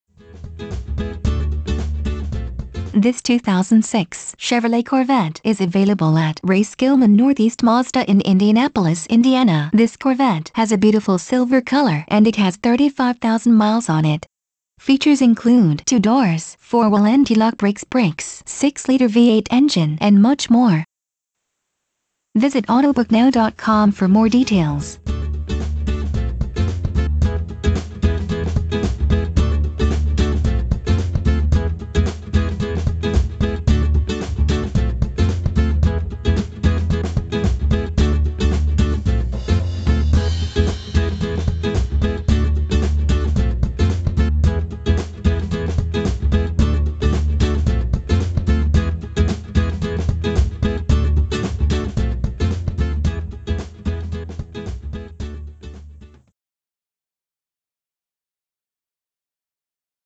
2006 Chevrolet Corvette Base in Indianapolis IN for Sale
2006 Chevrolet Corvette Base
Stock #: K1633
Price: $31,995
Miles: 35,370
Trim: Base
Body Type: Coupe
Color: Silver
Interior: Cashmere w/Highwear Nuance Perforated Leather Seat
Transmission: 6-Spd Paddle Shift w/Automatic Modes